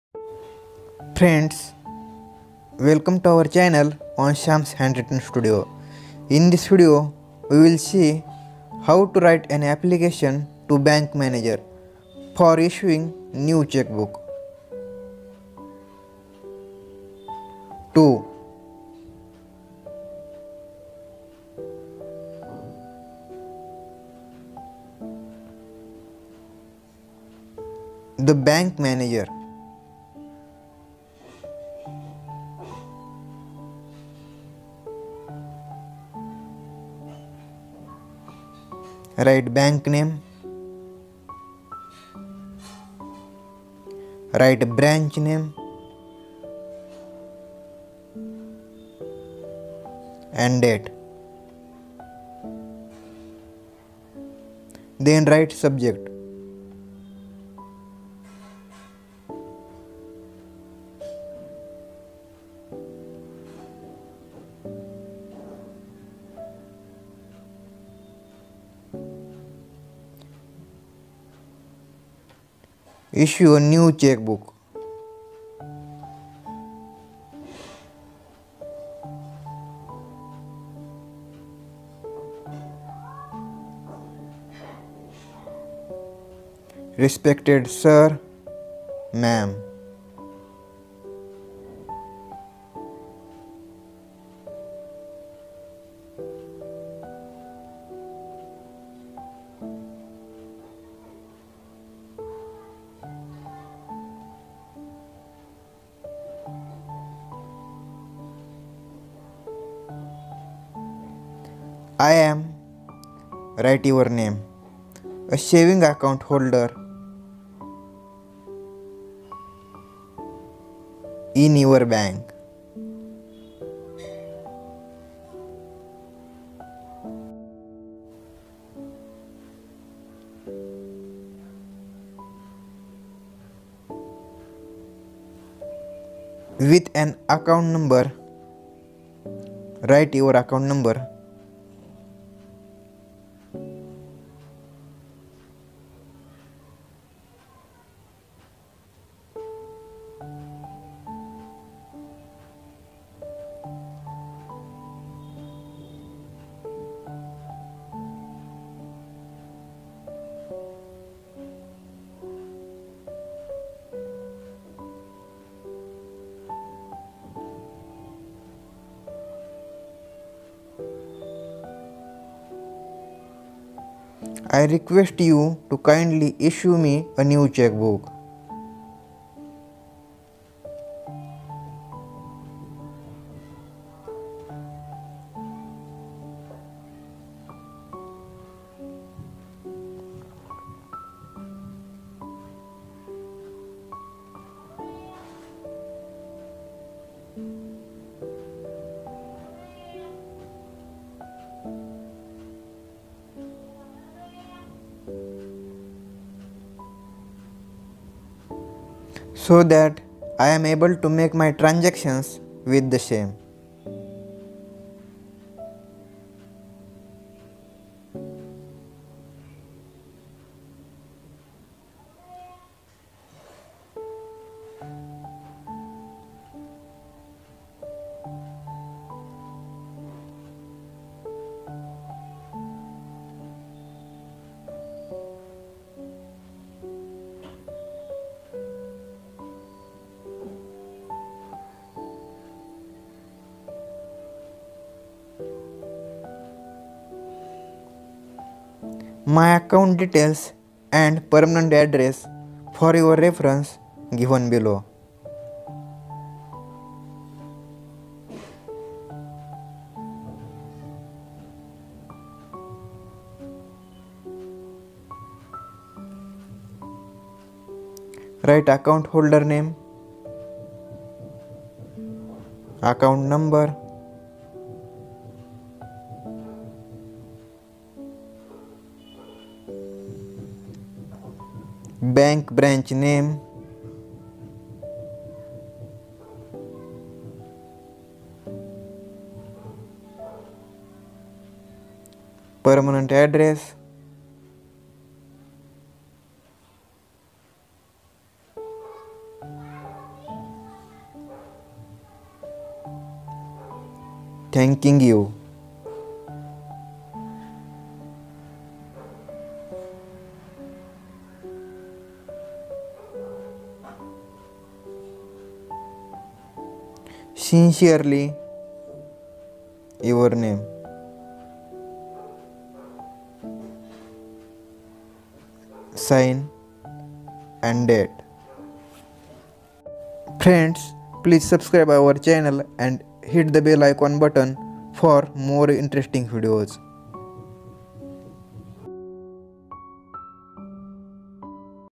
Write an application for new cheque book/letter writing for issue cheque book/letter to bank manager
Write an application for new cheque book l letter writing for issue cheque book l application to bank manager

Hello friends.
Welcome to our channel on Sam's handwritten studio..In this video we will learn how to write application to bank manager for issuing new Cheque book. Friends if you find this video helpful then please subscribe our channel and hit the bell icon button for the same...